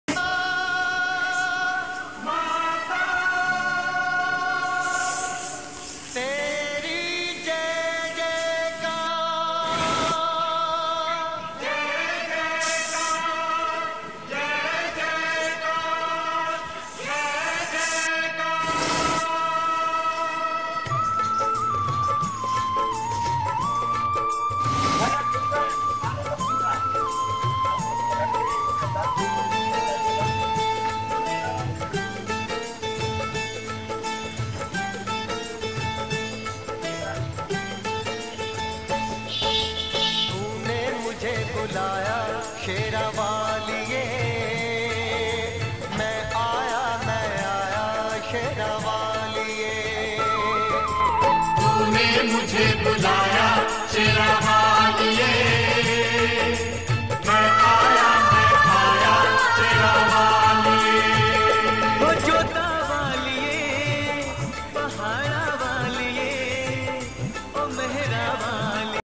Durga Mata
Hyderabad in Dhoolpet DurgaMata special idol made by Laxminarayana Kalakaar